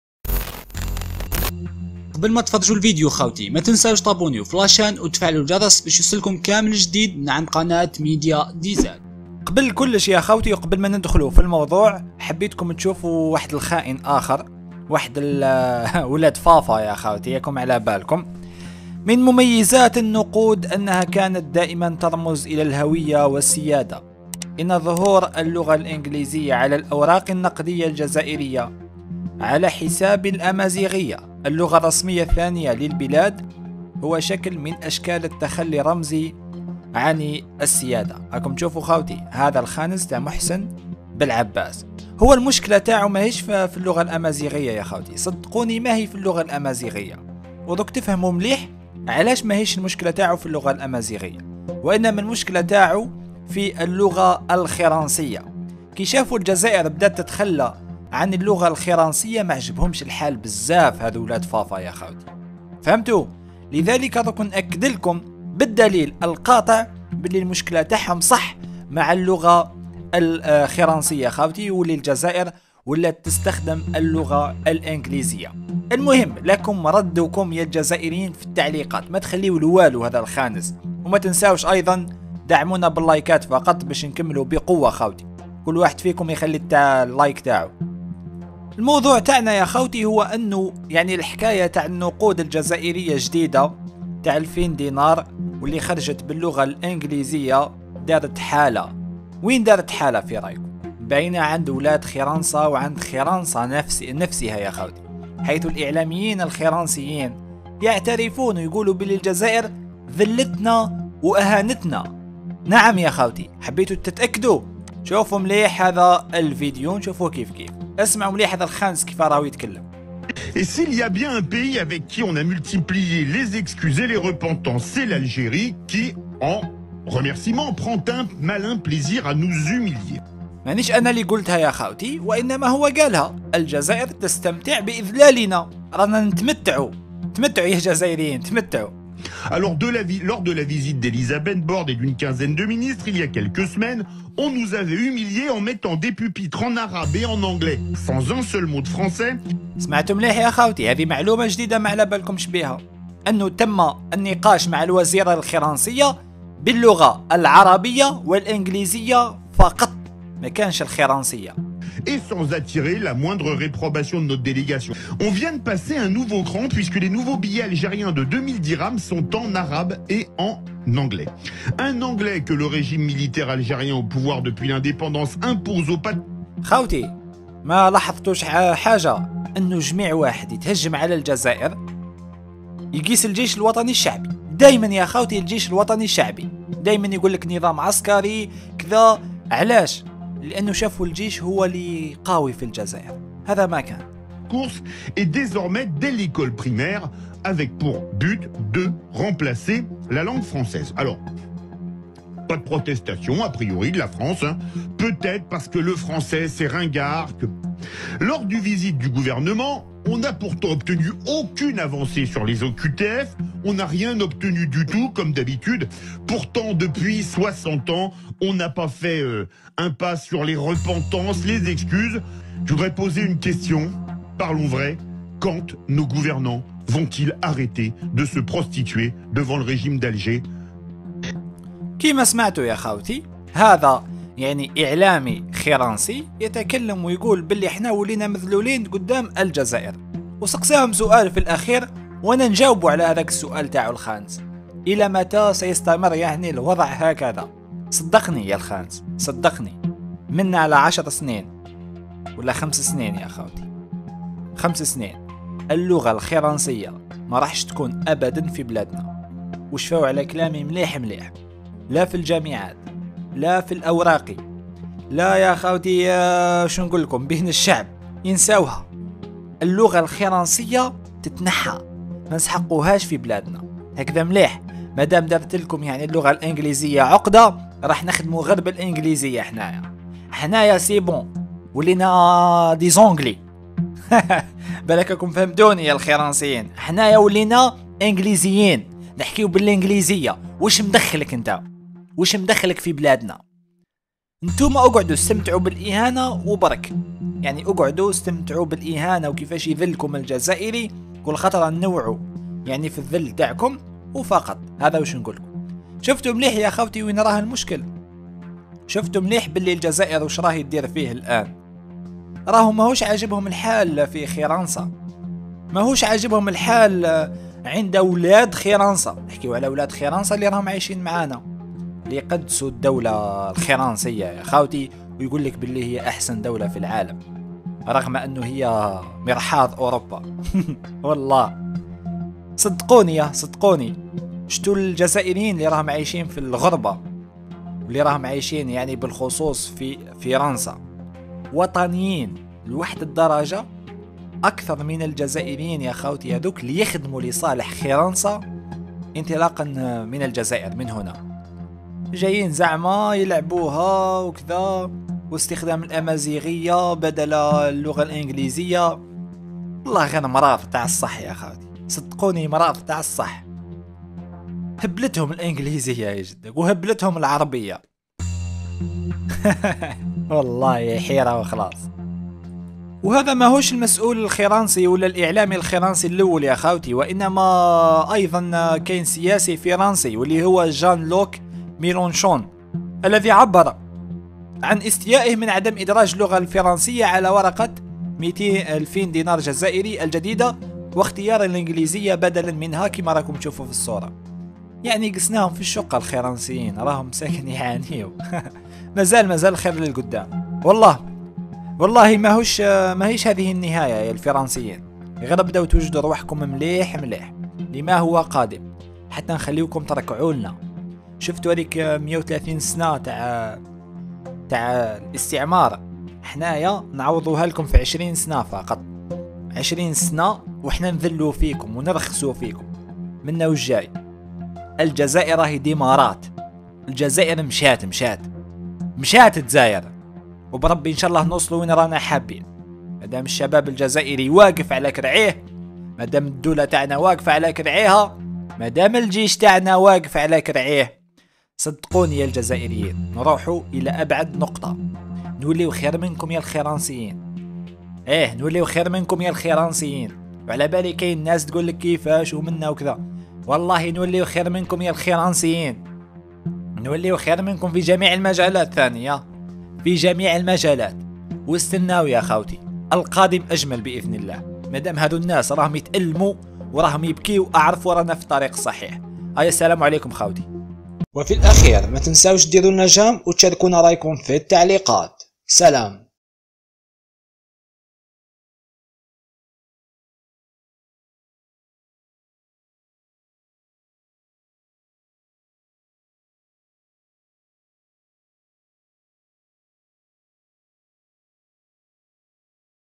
عـاجل..إعلامي فرنسي يبكي و هو يتسائل لماذا الجزائر تذلنا هكذا.. فجائه رد قوي من جزائري!
❤❤استغفر الله - اللهم صلي وسلم على سيدنا محمد ❤❤